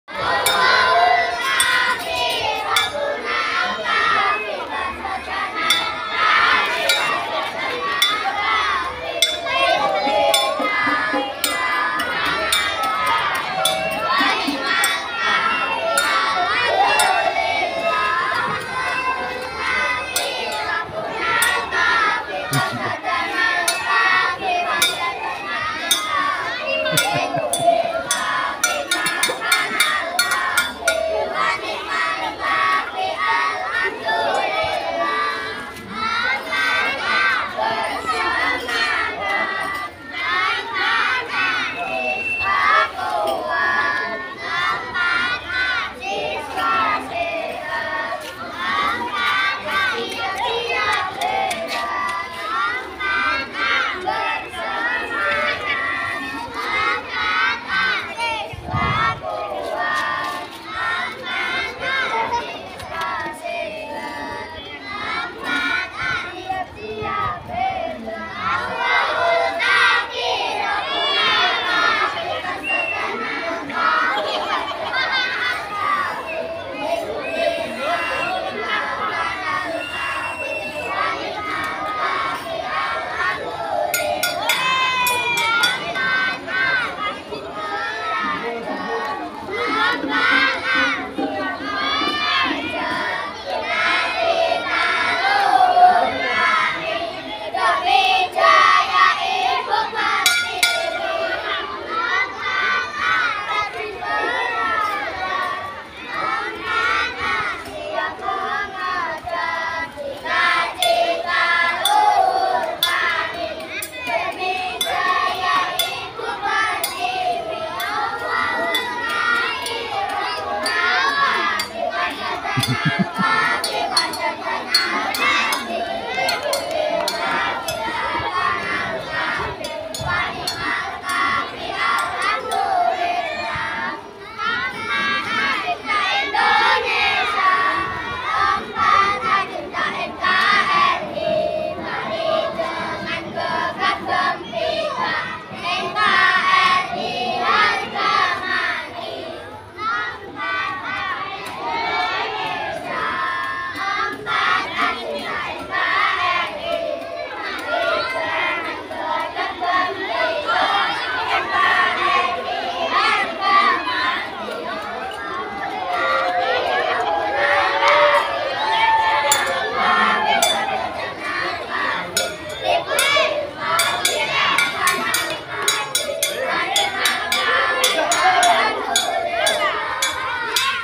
Yel-yel Kelas 4A
Dilaksanakan usai Upacara 17 Agustus 2024 (Sabtu)